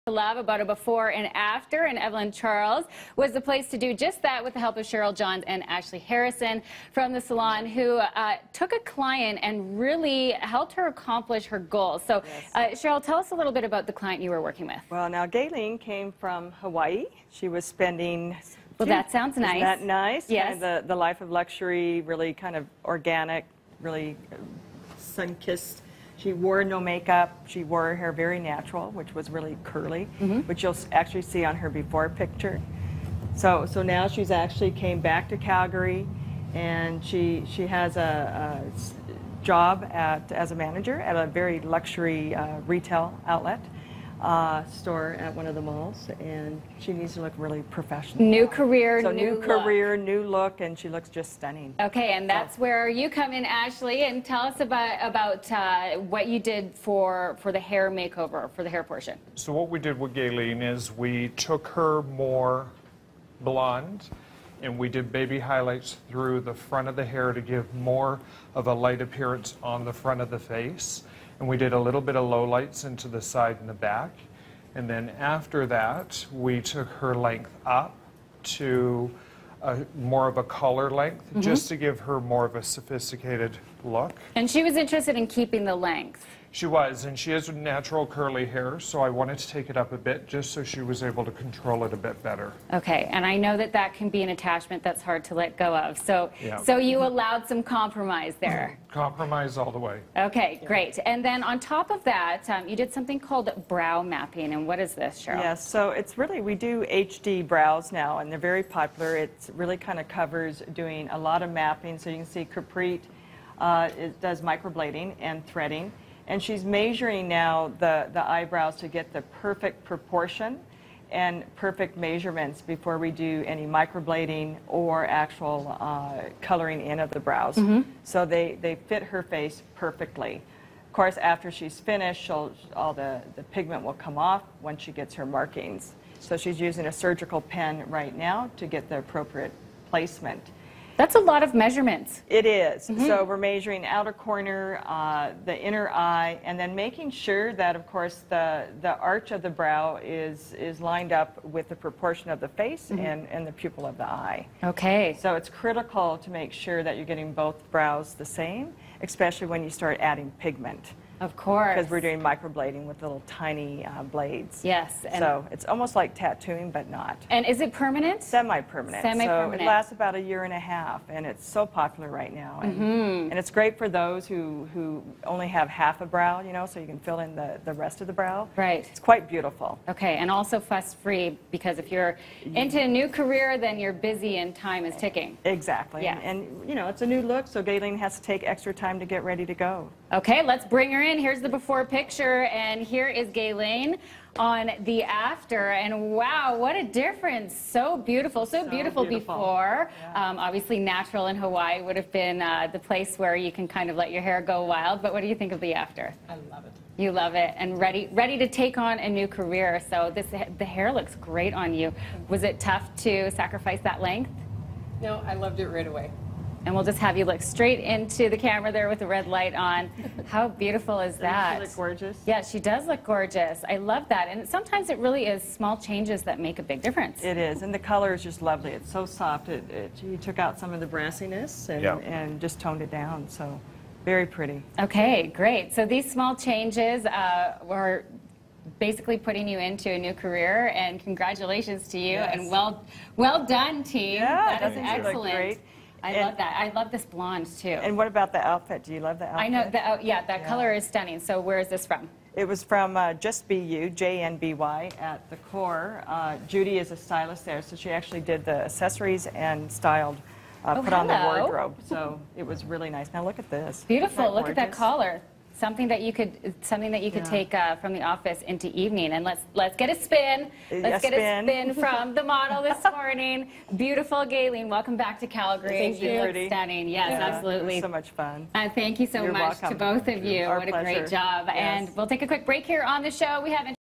Calgary career wardrobe makeover
As thousands of woman are pounding the Calgary pavement looking for work... Cheryl Johns from Eveline Charles is here to show us how to look spit and polish for your aspiring career.

Be the first to hear about City shows, online features and exclusive special offers.